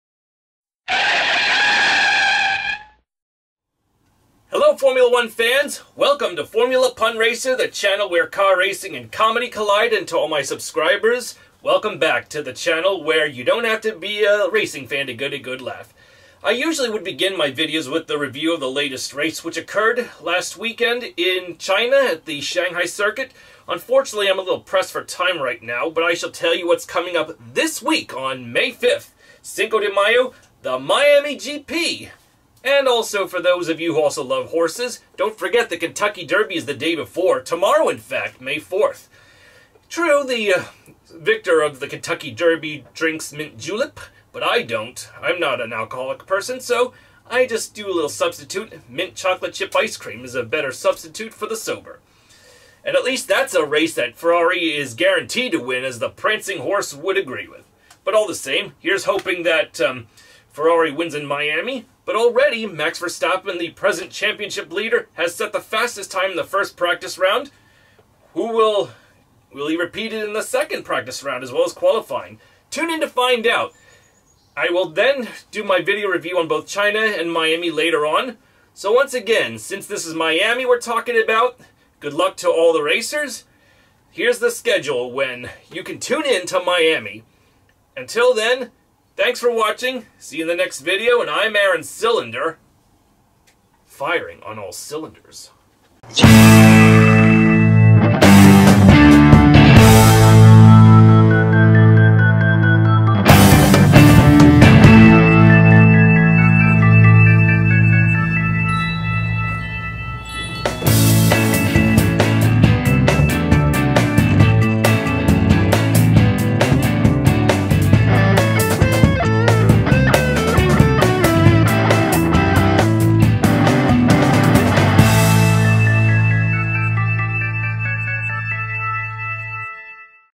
Miami Grand Prix-view
Even though I haven't yet started my review on the previous round of the chinesegp, courtesy of being kept busy, I offer you a filler of what's to come. And to let you know the upcoming round of the @Formula1 season takes place on May 5, Cinco de Mayo, in Miami, Florida. Hi, Lt. Horatio Caine. And yes, you can expect some csimiami one-liners when I review that race.